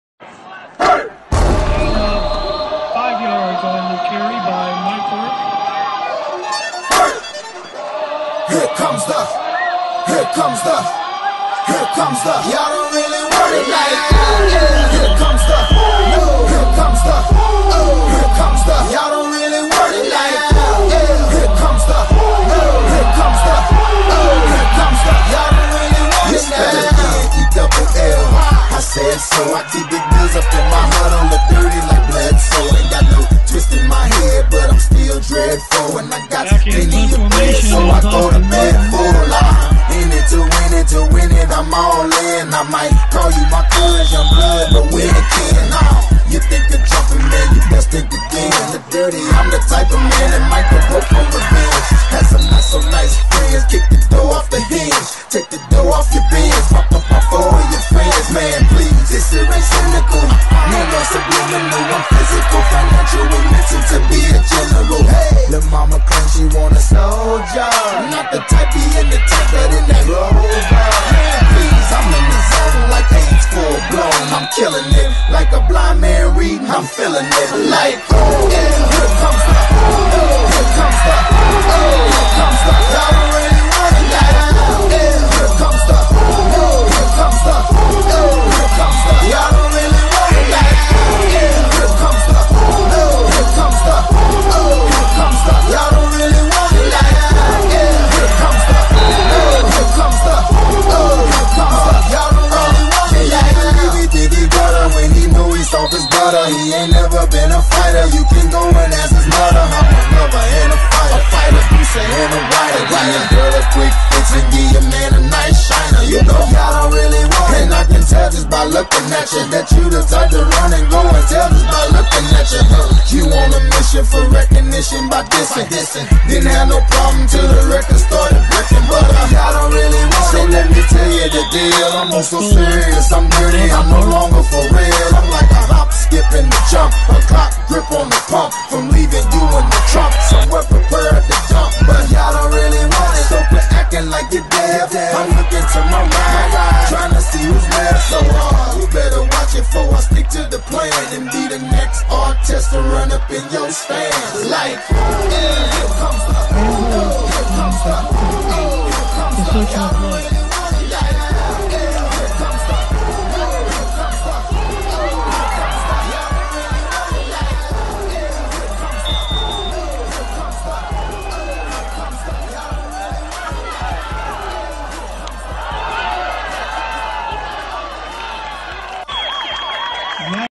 2013 Week #7- Maroa at Tuscola
The Warriors came out on the short end of the stick in Week #7 against a very talented Maroa-Forsyth team.